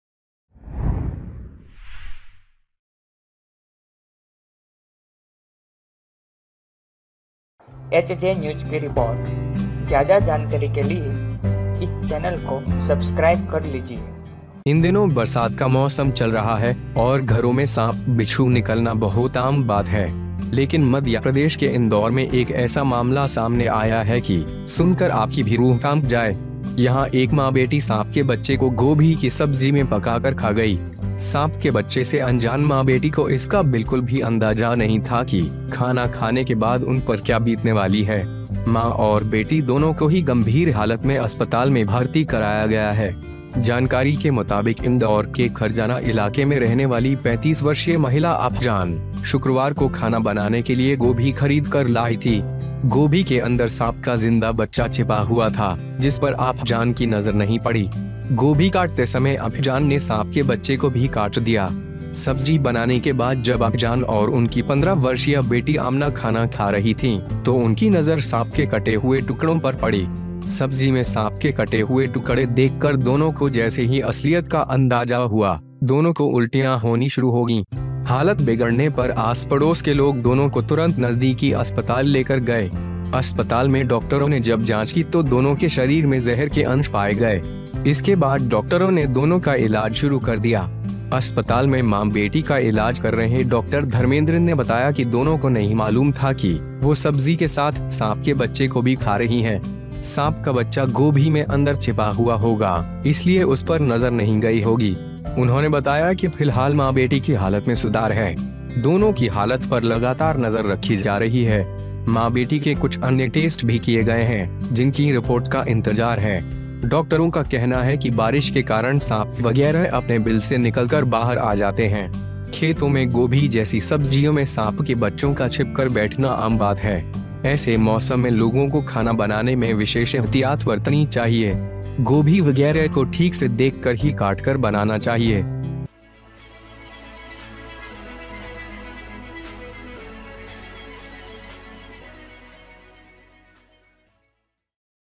गोभी के अंदर से निकला कुछ ऐसा जिसे देखकर हैरान रह जायेंगे आप
These days rainy season is going on, and it is very common to get snakes and scorpions in homes. But in Madhya Pradesh, one such case has been reported in Indore that, listening to you may be shaken. Here a mother and daughter cooked the snake's baby in cauliflower and cooked it. Unaware mother and daughter did not have any idea of ​​the snake's child, what is going to happen to them after eating them. Both mother and daughter have been admitted to the hospital in critical condition.

According to the information, 35-year-old Afzan, a resident of Kharjaana area of ​​Indore, brought cabbage to cook food on Friday. The baby inside the cabbage was hidden from the snake, which Afajan did not notice. At the time of cutting cabbage, Afzan also cut the snake's child. After making vegetable, when Afzan and his 15-year-old daughter were eating mamaa meal, they were caught on the cut pieces of snake.

After seeing the pieces of snake in the vegetable, both of them realized the reality, both of them started getting vomiting. When the condition deteriorated, the neighbors immediately took both of them to a nearby hospital. When the doctors examined the doctors in the hospital, the contents of the poison were found in both the bodies. After this the doctors started treatment for both of them.

Doctor Dharmendra, who was treating mother-daughter in the hospital, told that both did not know that he was eating snakes with the vegetable. Snake child must be hidden in the cabbage, so he has not been seen. He said that the condition of mother-daughter is currently improved. The condition of both are constantly being monitored. Some other tests of mother-daughter have also been done, whose reports are awaiting.
Doctors say that due to rain the snakes come out of their bills and come out. It is common for saplings to sniff in the vegetables like cabbage in fields. In such weather, people should take special precautions to make food. Cauliflower should be made by cutting and looking only.